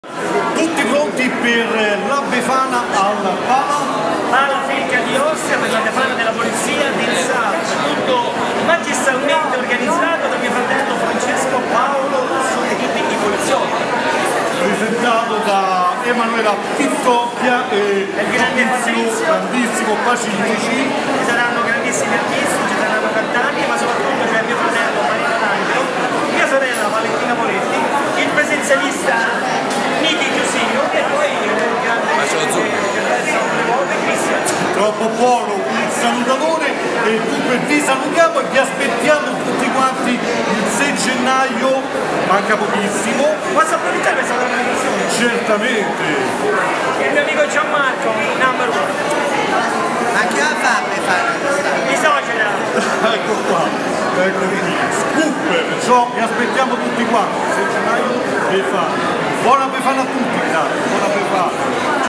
LA BEFANA IN POLIZIA SAP 2015 : Enzo Salvi Vi invita con Valentina ,Mariano ,Niki e il Salutatore
Enzo Salvi Vi invita e aspetta alla BEFANA IN POLIZIA SAP il 6 Gennaio 2015 a Ostia al Palafjilkam con Mariano D'angelo,Valentina Paoletti,il Disturbatore e il Salutatore ci saranno tantissimi Artisti che vi aspettano IL TUTTO ORGANIZZATO DA FRANCESCO PAOLO RUSSO Per inviti gratuiti